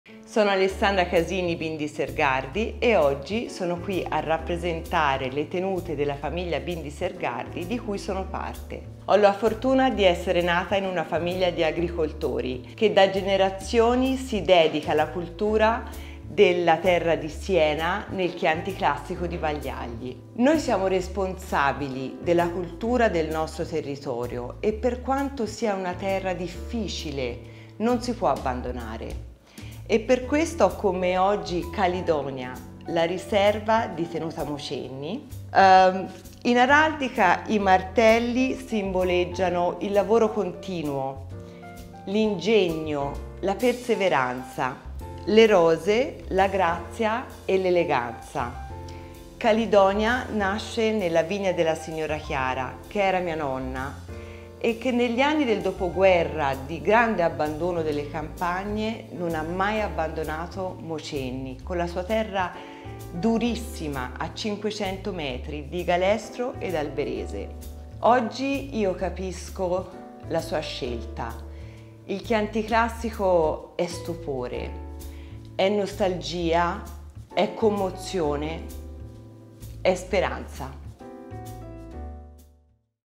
Bindi Sergardi
“Ho la fortuna di essere nata in una famiglia di agricoltori, che da generazioni si dedica alla coltura della terra di Siena, nel Chianti Classico di Vagliagli. Oggi ho con me Calidonia, la Riserva di Tenuta Mocenni. In Araldica i martelli simboleggiano il lavoro continuo, le rose invece la grazia e l’eleganza. ”

“I was lucky enough to be born into an agricultural family which for generations has been cultivating the land around Siena, in the Chianti Classico zone of Vagliagli. I've got Calidonia with me today, Tenuta Mocenni's Riserva wine. In Araldica, hammers symbolise continuous work, whereas the rose is for grace and elegance. ”

Azienda / Estate: Bindi Sergardi
Intervistato / Interviewee: Alessandra Casini Bindi Sergardi